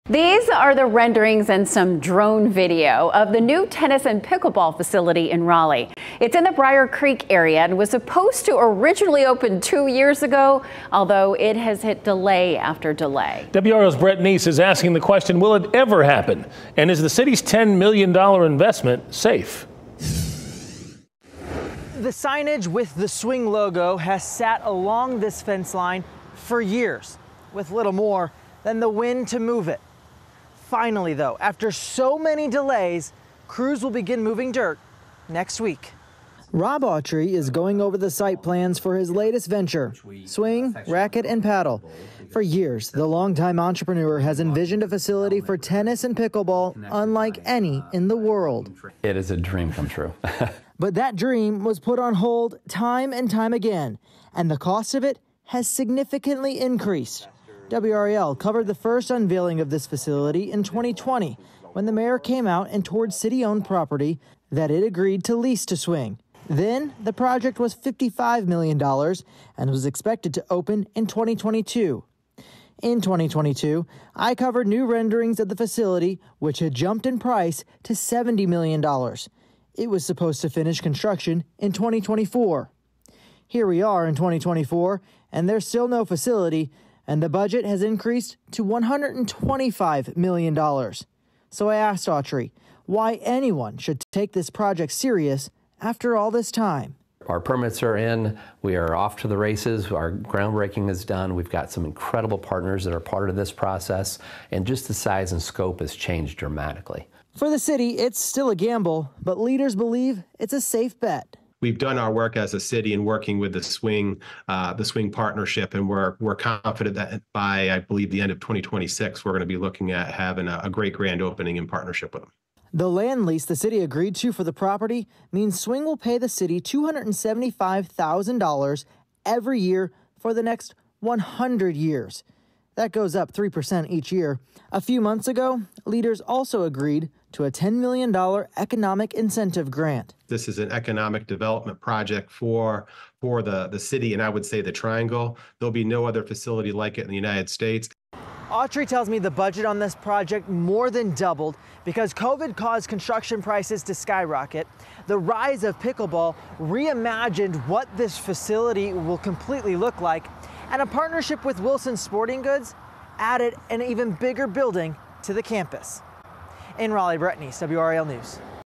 $125 million tennis and pickleball facility to open by 2026 in Raleigh
A new pickleball and tennis facility in Raleigh's Brier Creek area is projected to cost $125 million and is expected to open by the end of 2026.